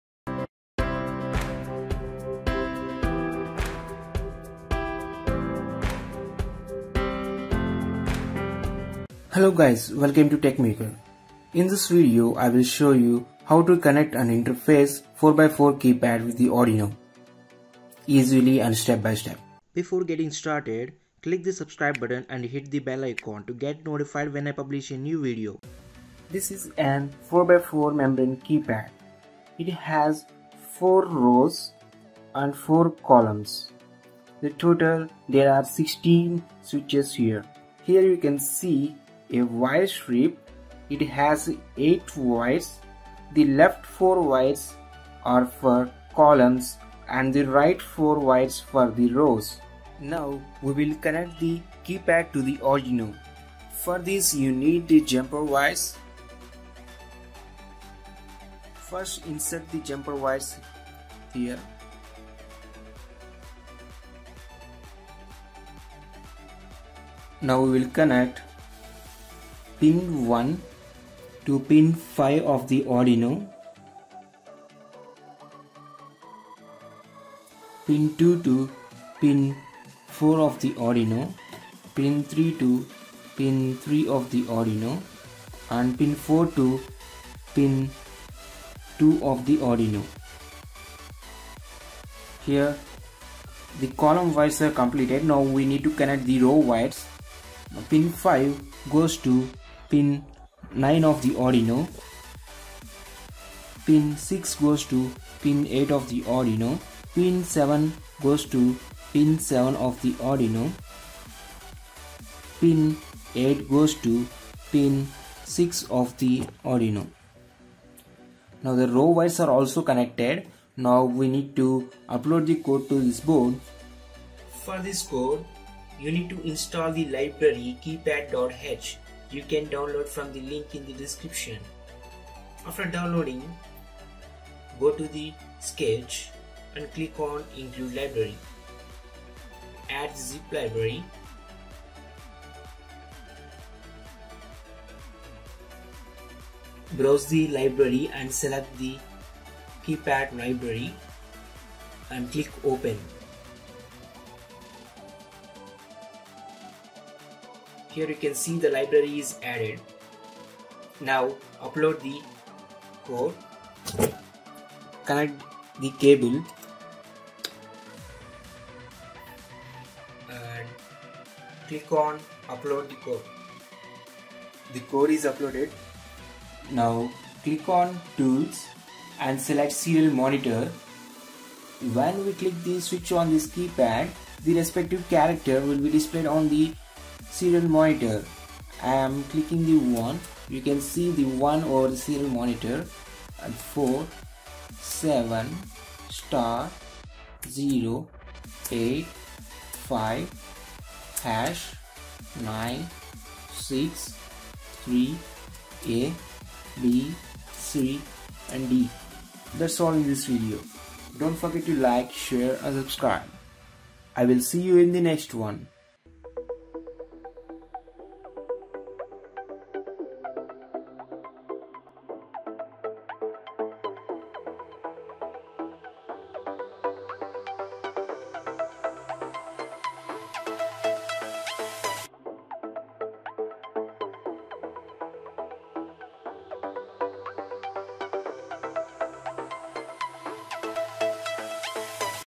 Arduino 4x4 Matrix Keypad Tutorial | Keypad library for Arduino
In this video, I will show you how to interface 4x4 keypad with Arduino Uno easily and step by step.

Buy Parts~

Amazon India

Links~~